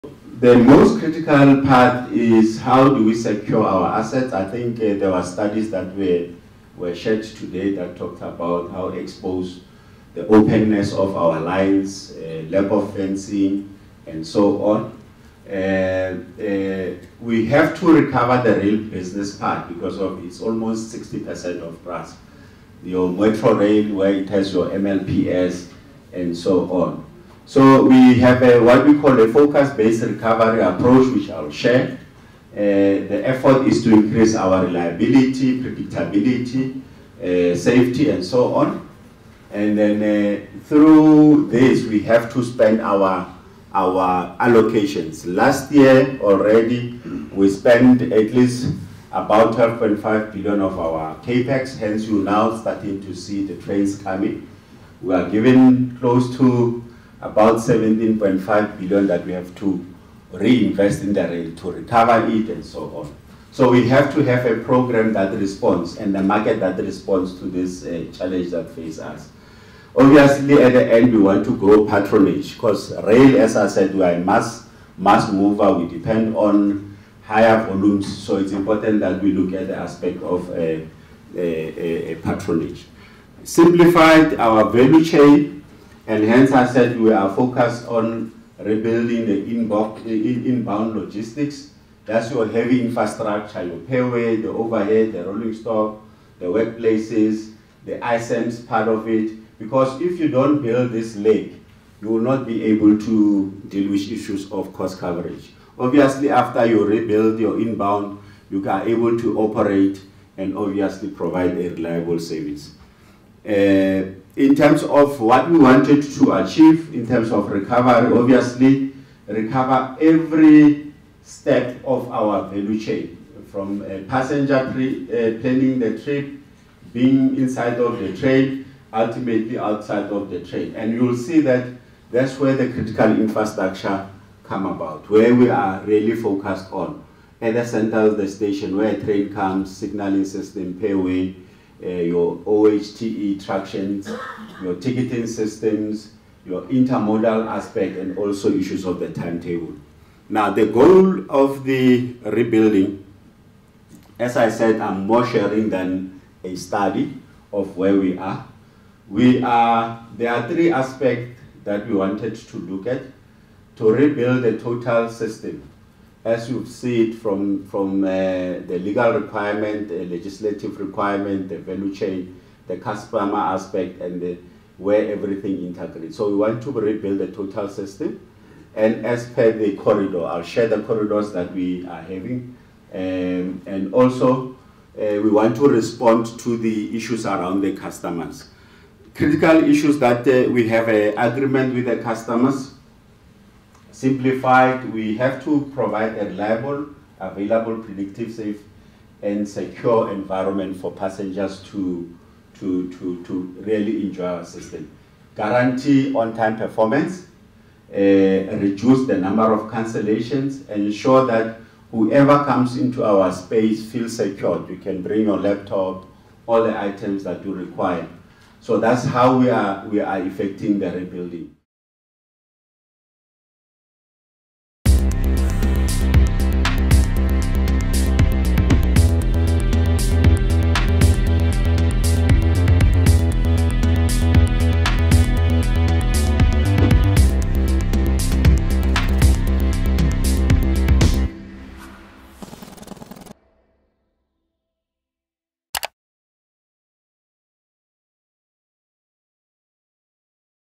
PRASA undertaking recovery programme
Passenger rail operator the Passenger Rail Agency of South Africa (PRASA) has changed its approach to build from the ground up and is implementing a five-point plan as part of its recovery efforts, Gauteng regional operations manager Abram Siliki Nkgabutle has outlined.